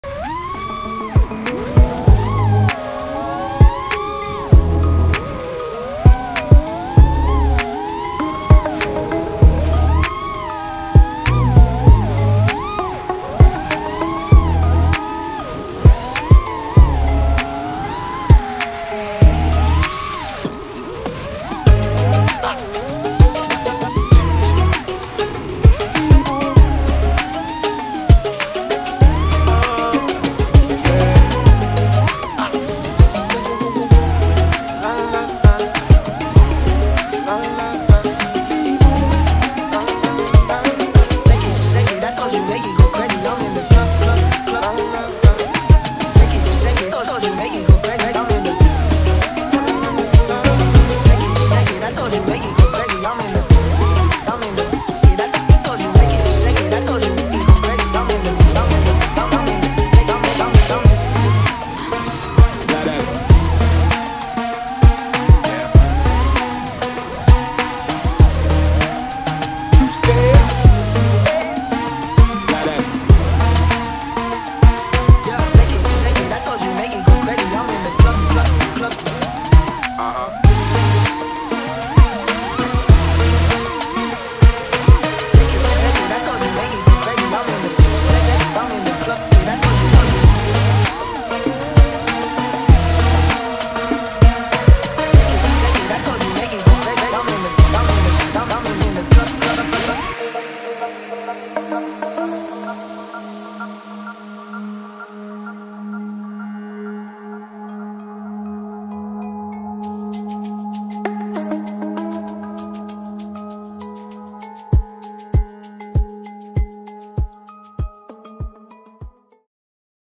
First Freestyle Session with the Thumb Grippies (These leveled up my proxy!)
I just got my Thumb Grippies in for my Taranis, and decided to give them a whirl with a little freestyle at a new spot.  I am thoroughly impressed with them!  So much grip now, and super comfortable!  My only regret is not switching sooner.

Go get some!

Color Grade:
Johnny FPV LUTs